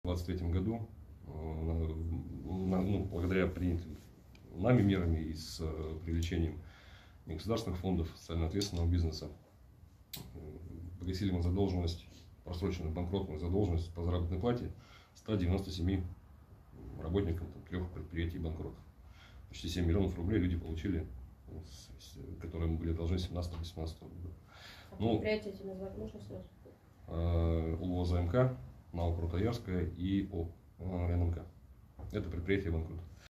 Владислав Беспалов
В 2023 году более 7 миллионов рублей получили работники обанкротившихся ранее предприятий в Назарово и Назаровском районе